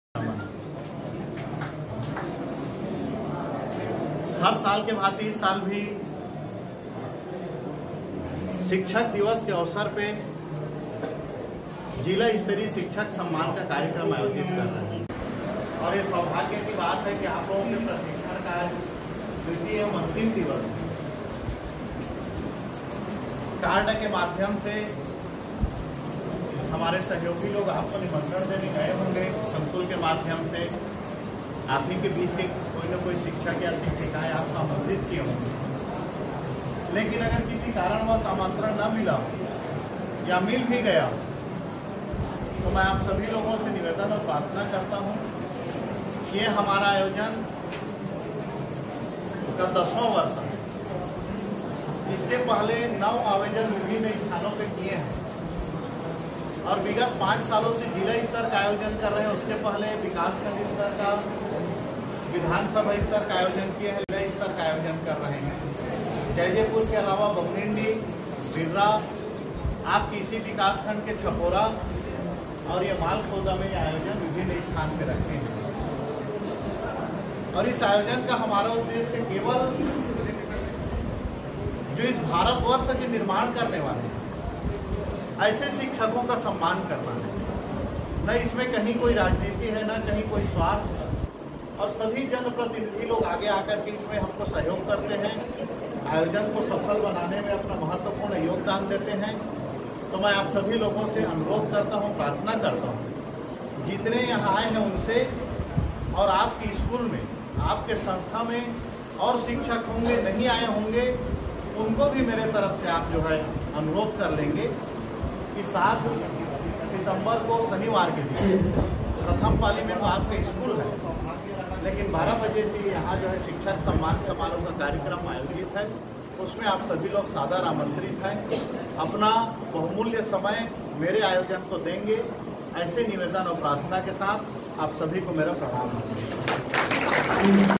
सप्रेम आमंत्रण (केशव चंद्रा विधायक जैजैपुर)
आप समस्त शिक्षक सादर आमंत्रित है 07 सितम्बर 2019 को दोपहर 12 बजे से हाई स्कूल मैदान मालखरौदा में